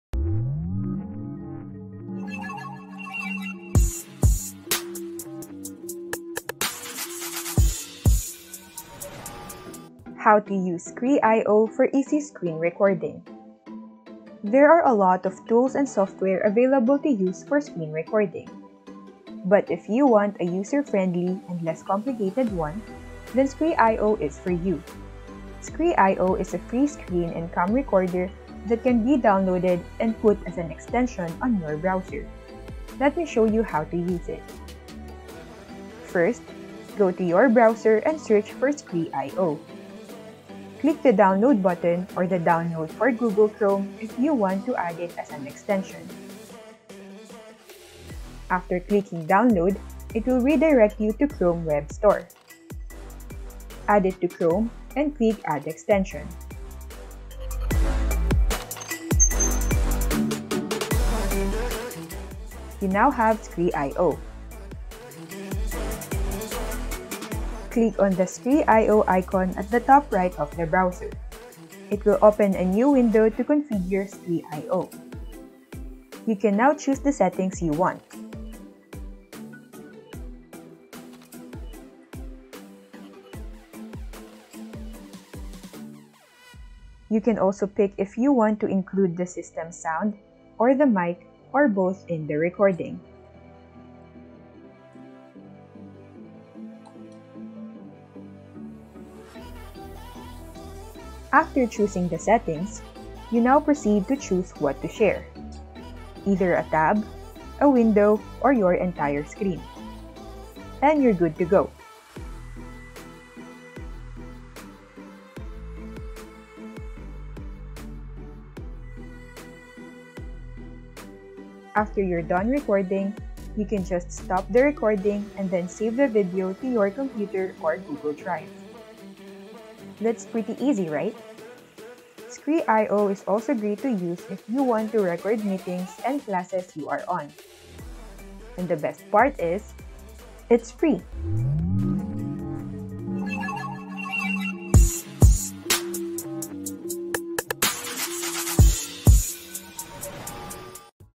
SCRE.IO: The Ultimate Tutorial on How to Screen Record for FREE
There are hundred of tools and software online that are available to use for screen recording but if you want a free, no hidden charges, user-friendly, and less complicated one then scre.io is for you.

Scre.io is a free screen and cam recorder that can be downloaded and put as an extension on your browser. Learn in this video tutorial on the step-by-step guide on how to use it.